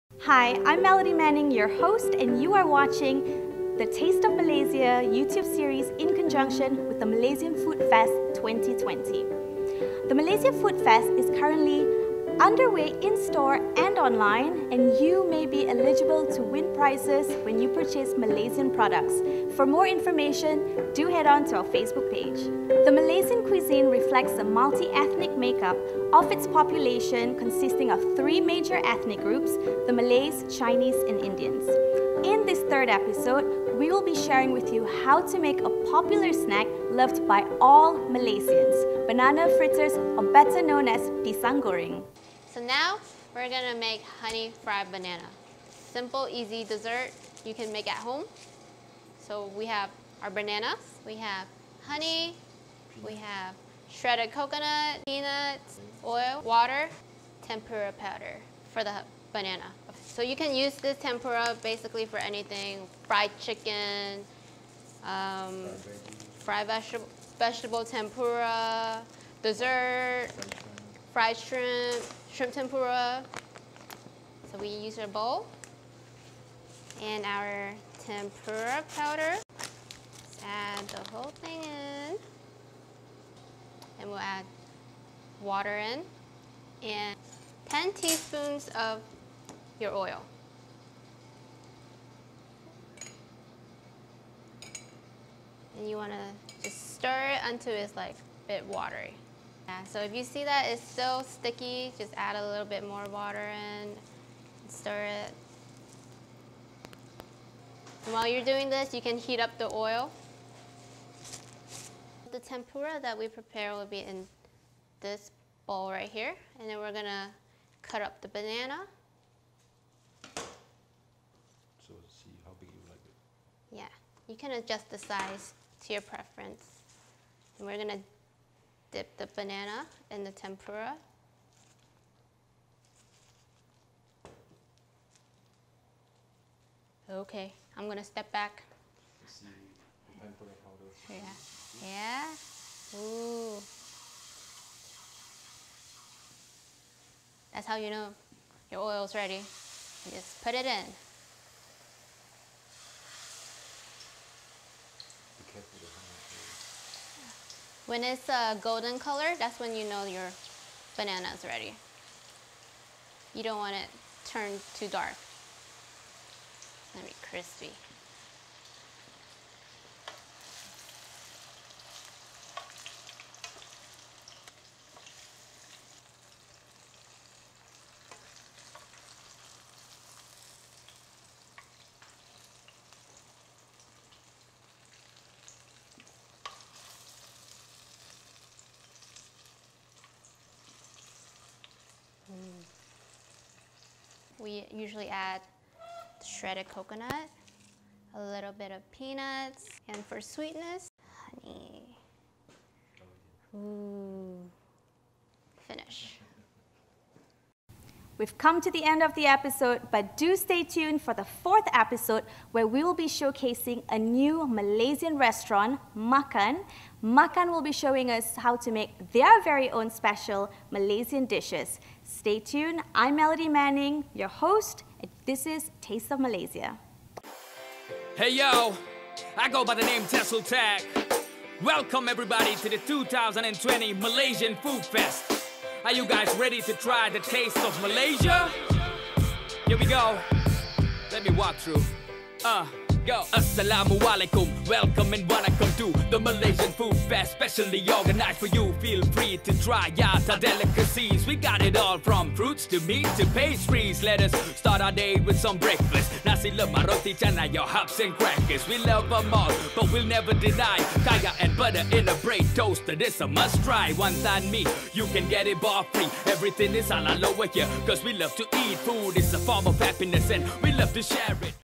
Malaysia Food Fest 2020 : Episode 3 - Banana Fritters (Pisang Goreng)
Chef Tony Goo of the Satay Club makes a simple but delicious dessert, Pisang Goreng. Crunchy on the outside but soft and sweet in the middle.